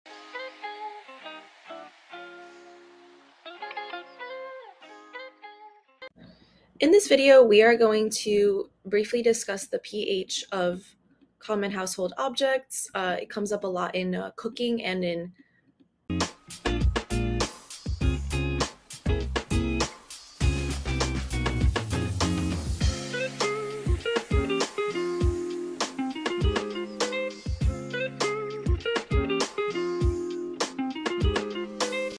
Write a hypothesis about the pH level of common household solutions based on what you know about th…
Write a hypothesis about the pH level of common household solutions based on what you know about their physical and chemical properties. Use the format of    quot;if . . . then . . . because . . .â€   and be sure to answer the lesson question    quot;How is pH used to determine if a solution is acidic or basic?â€